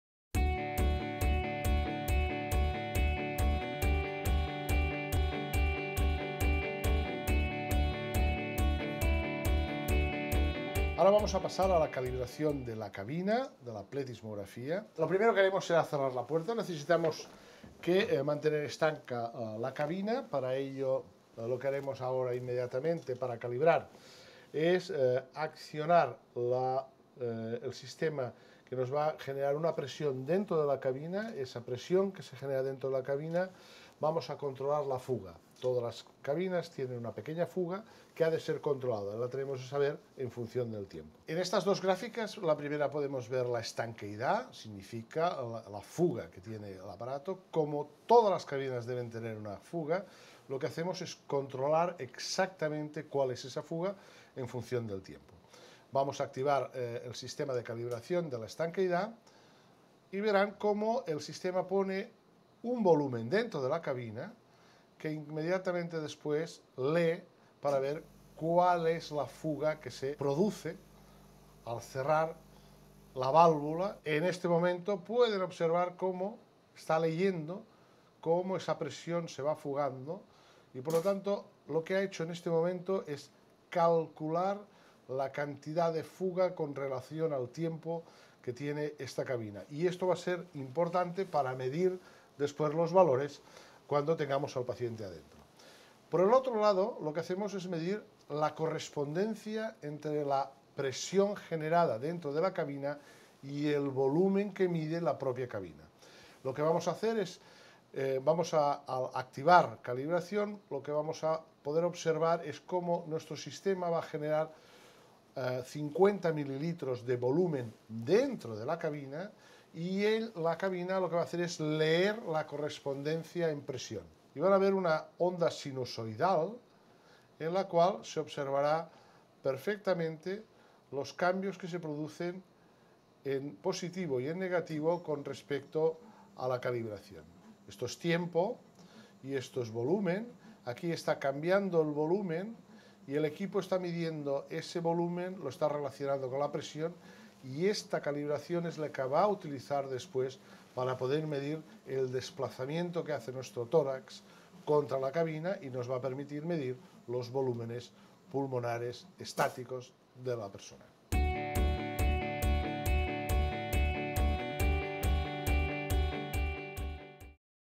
Volúmenes Pulmonares por pletismografía corporal. Calibración de cabina
Vídeo correspondiente al Módulo 3"Espirometría y otras pruebas funcionales respiratorias". del Programa Formativo Acreditado sobre EPOC.